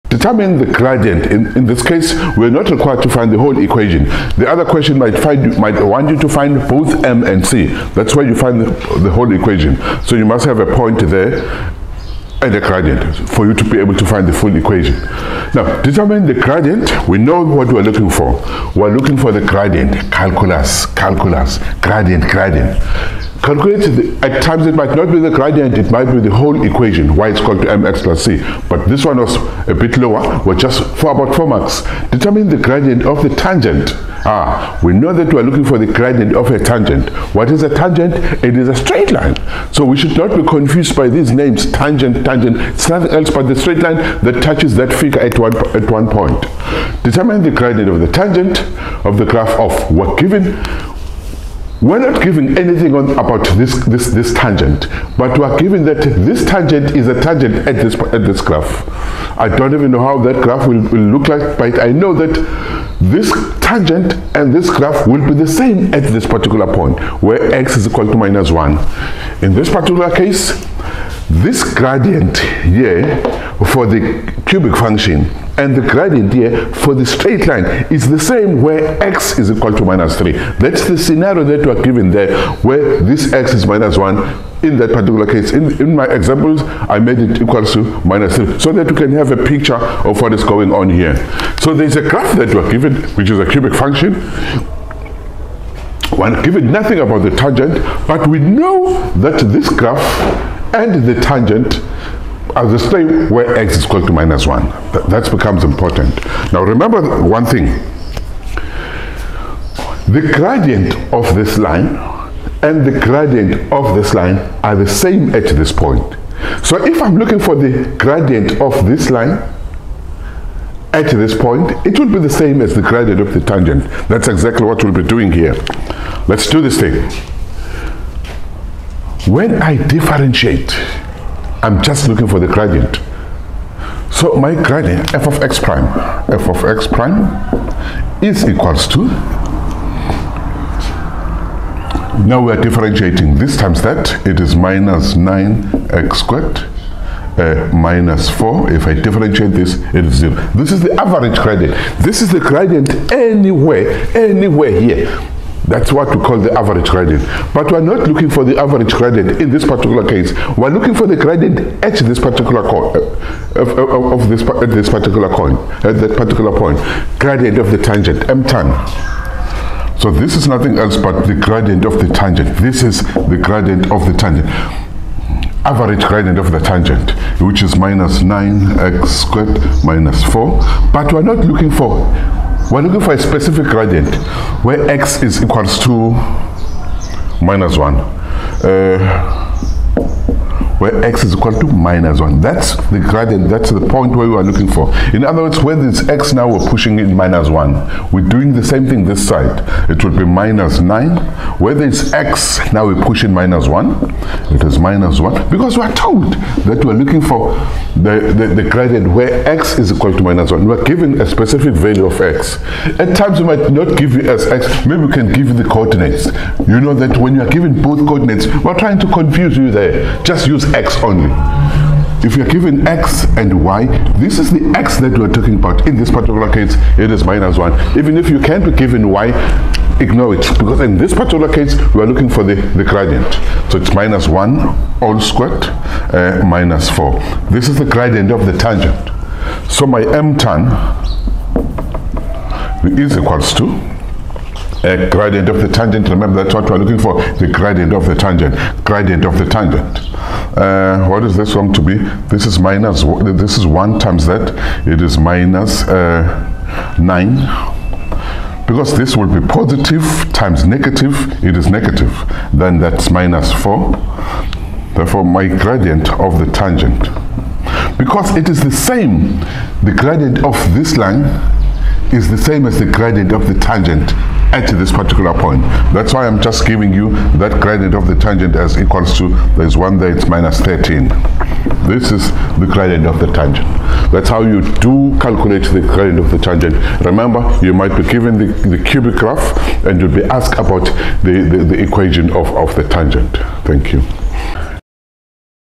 Calculus | Exam Prep – VirtualX MasterClass | Series 10
Dive into "Calculus | Exam Prep – VirtualX MasterClass | Series 10" as we delve into Grade 12 calculus principles, focusing on finding the gradient of a tangent on a graph. Enhance your understanding of differentiation and prepare effectively for your exams.

🌟 Series Highlights:
• Tangent Calculation Mastery: Learn how to determine the gradient of a tangent on a graph.
• Exam Focused Review: Understand the process of differentiation to calculate the gradient.
• Real-World Application: Apply concepts through practical examples, analyzing specific points on graphs.
• Step-by-Step Guidance: Follow detailed steps to accurately compute the gradient of tangents, reinforcing your exam readiness.

Don't miss this opportunity to enhance your calculus skills and gain a competitive edge in understanding how to calculate the gradient of tangents. Watch "Calculus | Exam Prep – VirtualX MasterClass | Series 10" and become proficient in solving calculus problems!

Associated Tags